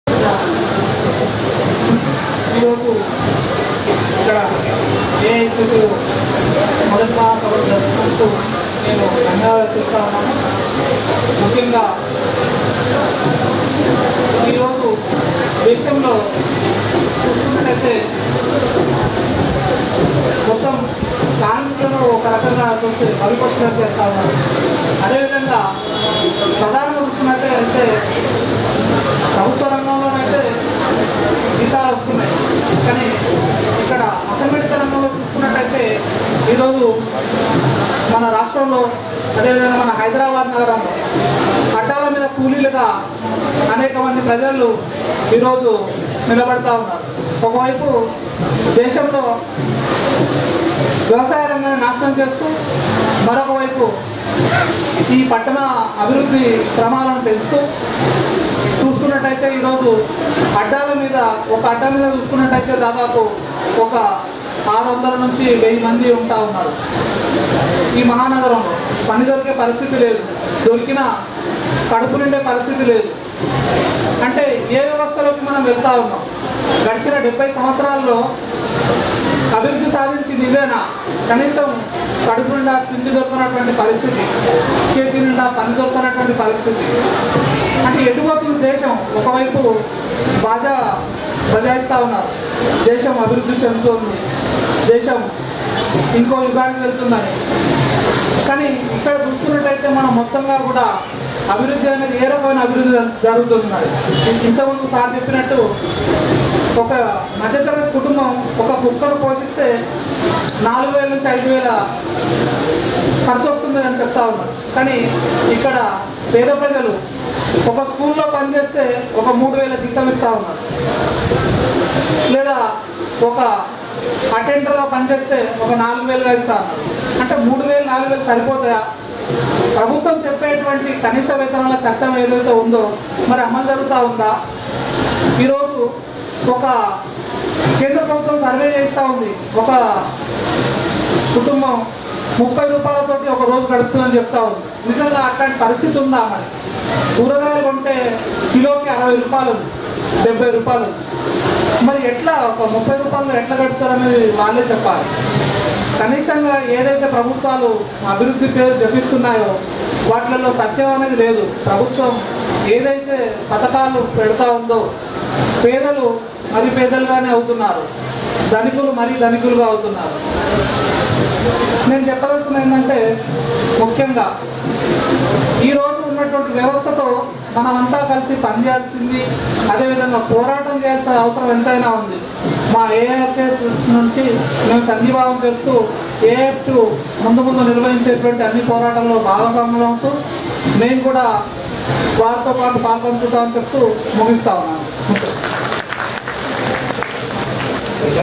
AIFTU 26th State Conference | Janavaninews
All India Federation of Trade Unions (AIFTU) will celebrate its 26th anniversary on August 9th 2019. After formation of Telangana State, this is the first conference of the AIFTU. AIFTU held its formation meeting at Mumbai in 1993 and first conference was held in 1994 in Hyderabad.